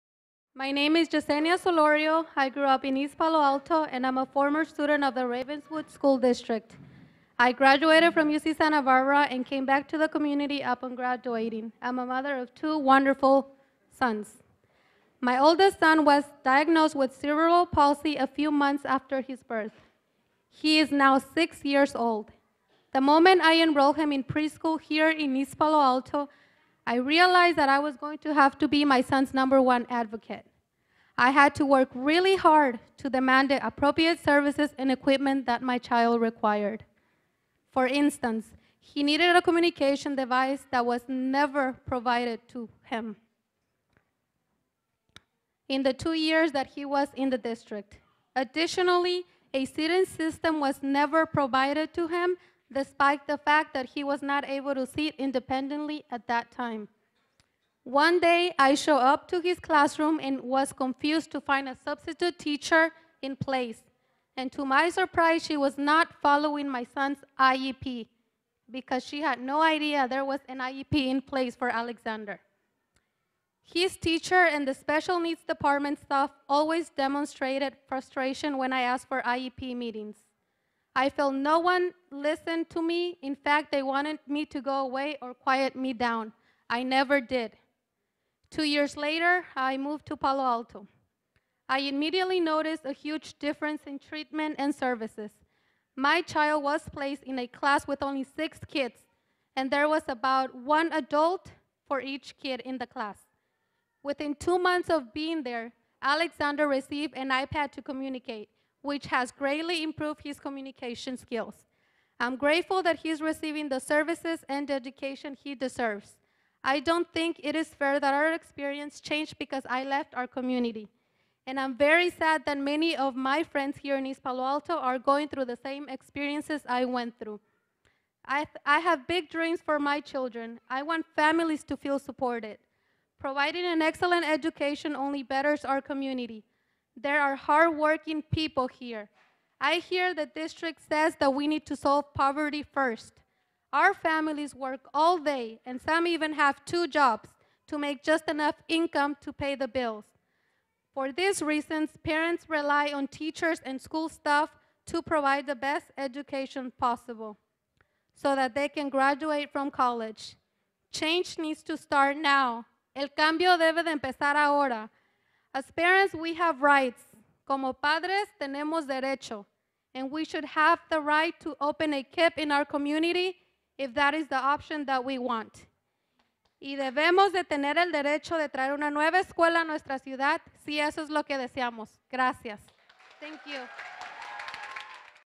EPA Parent Leader Jessenia Soloria Gives Testimonial
East Palo Alto Parent Leader Jessenia Soloria gives a testimonial on her experience with public school education in the Ravenswood School District.